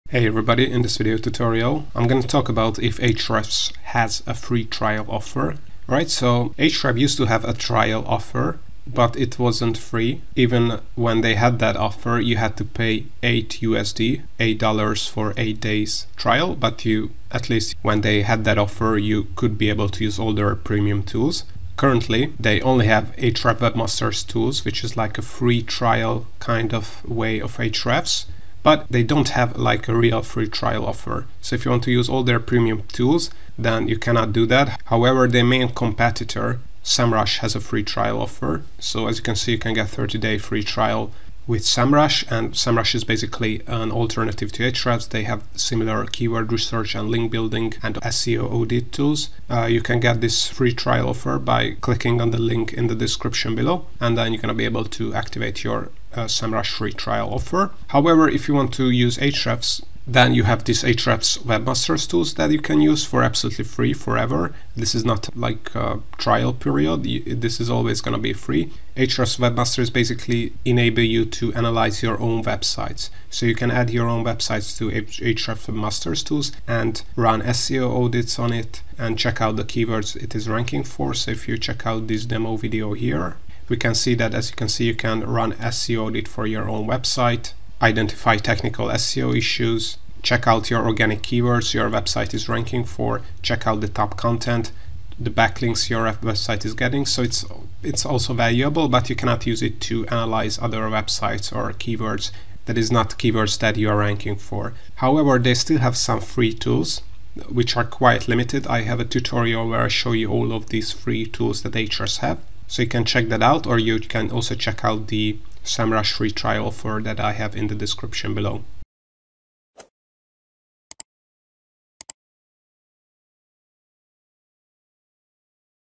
Ahrefs Free trial & Alternatives [in 2025]
Learn if Ahrefs has a free trial, how to use Ahrefs for free and what Ahrefs alternative has a free trial offer that you can claim.